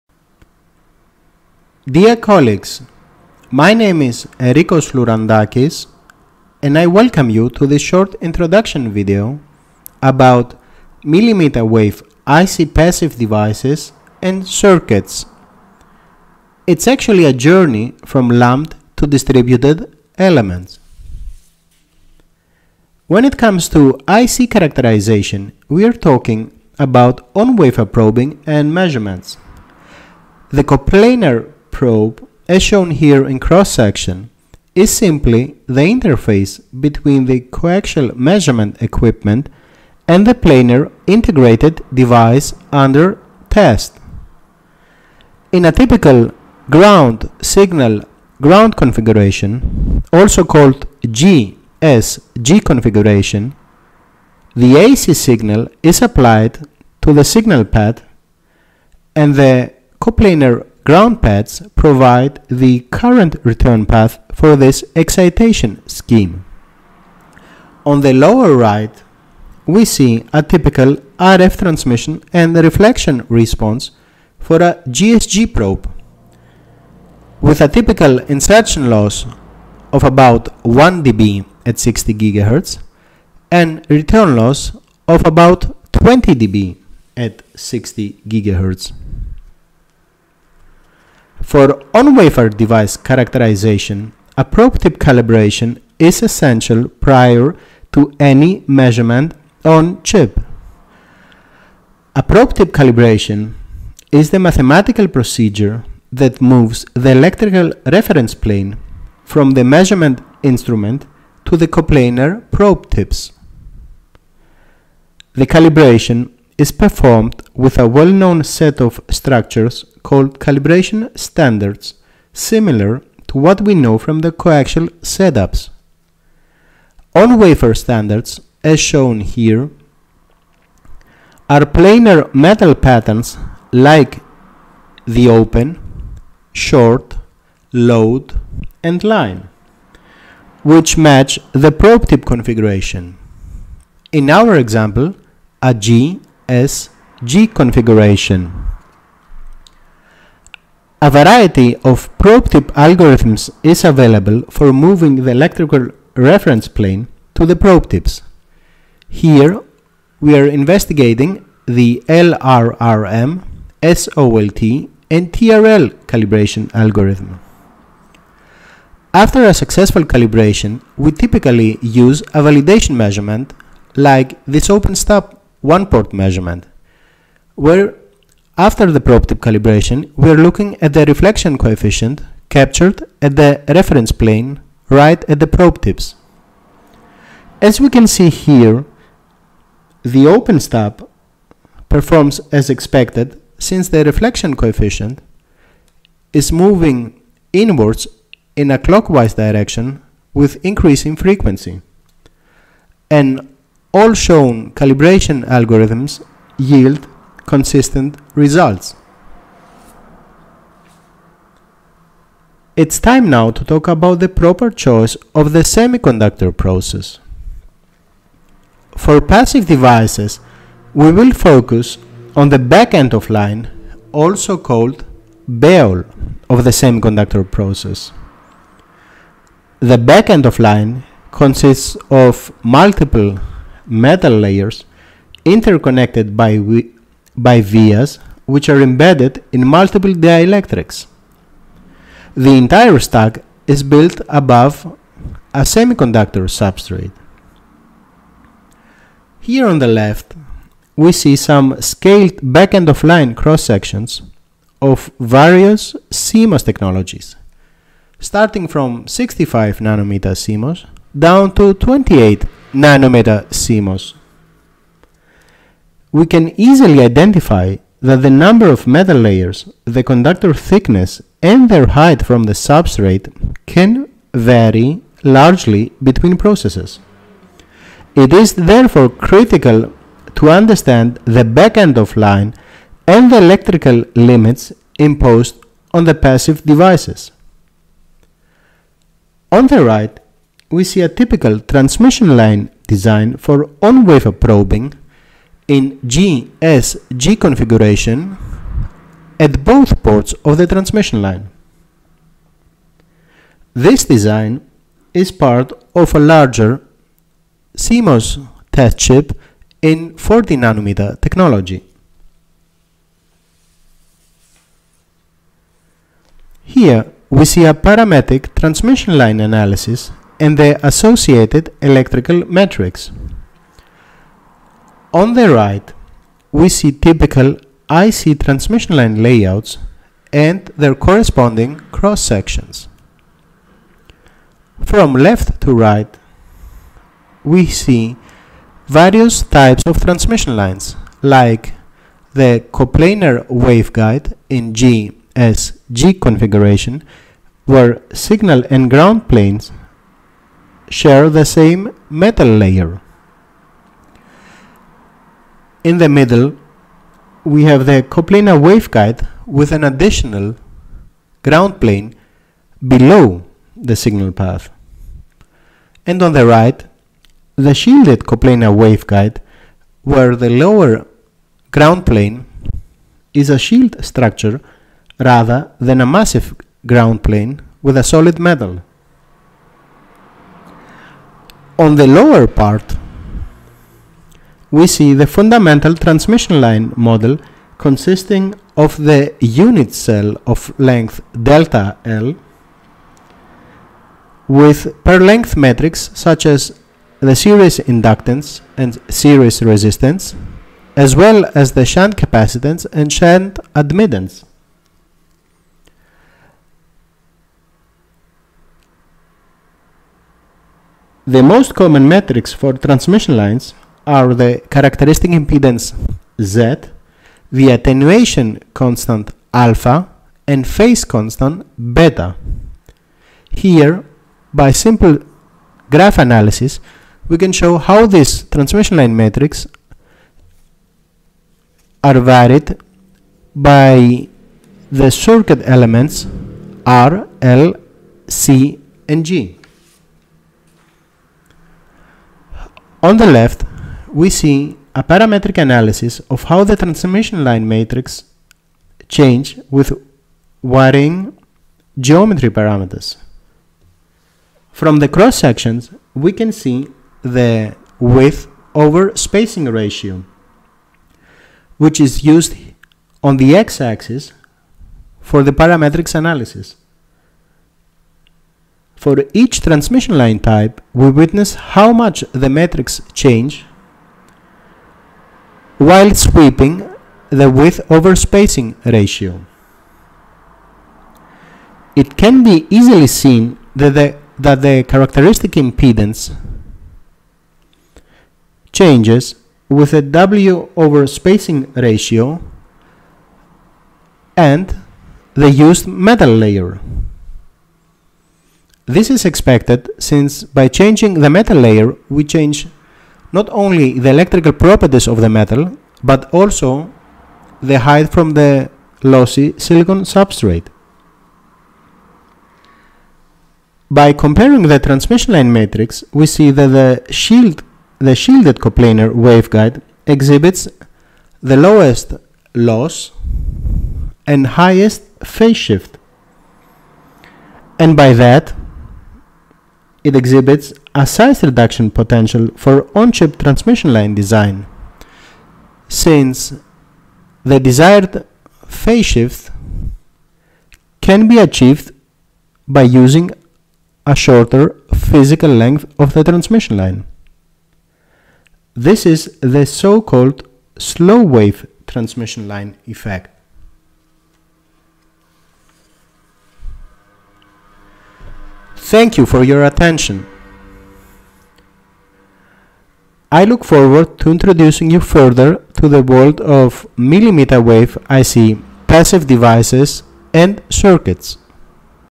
CMOS mm-Wave Transmission Lines
This paper focuses on nm CMOS transmission line design as distributed passive elements and their application in mm-wave integrated circuits. A variety of transmission lines such as coplanar waveguides (CPWs), shielded coplanar waveguides (SCPWs), and CPW with ground are analyzed in terms of their geometry and electrical properties.